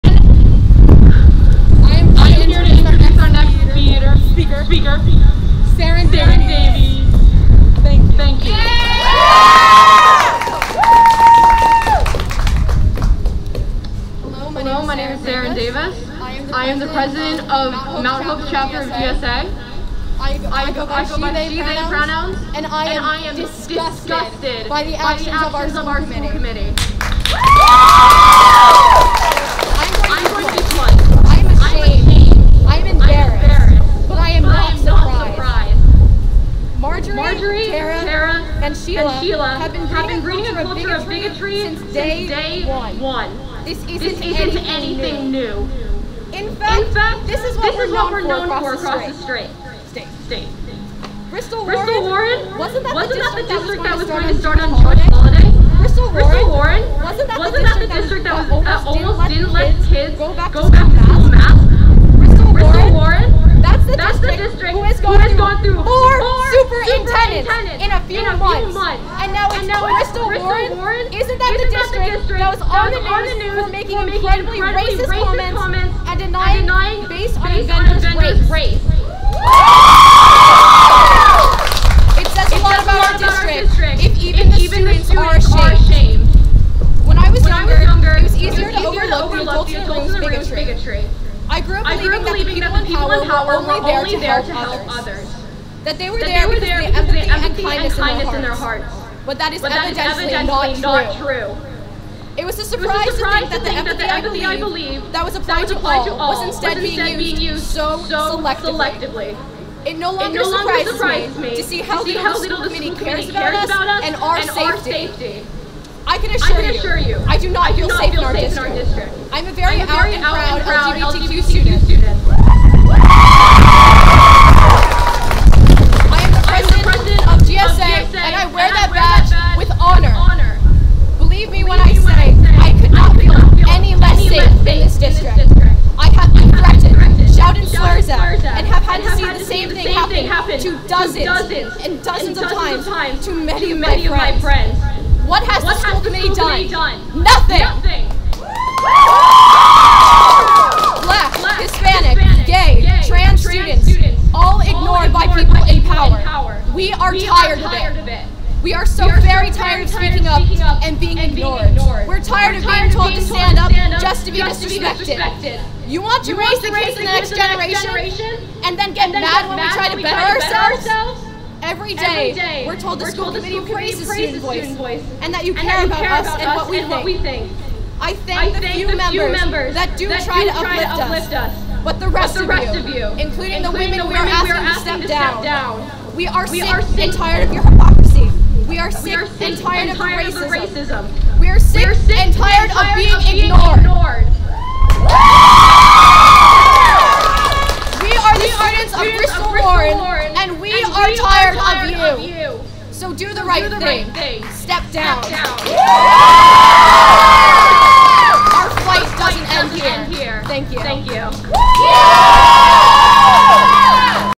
Student Leader Calls Out Bristol Warren School Committee’s Actions
Students in Bristol are calling for the resignation of three Bristol Warren School Committee members following their controversial vote against funding a teacher training program led by an educator experienced in supporting Black and brown students. Seren Davies, president of Mt. Hope’s GSA chapter, spoke out during a recent meeting, expressing frustration and demanding accountability. The Mt. Hope Student Union has labeled the decision as racist, citing a lack of support for inclusive education. Many students have drawn inspiration from youth-led campaigns by the Providence Student Union, fueling their determination to bring Gen Z perspectives into the school system.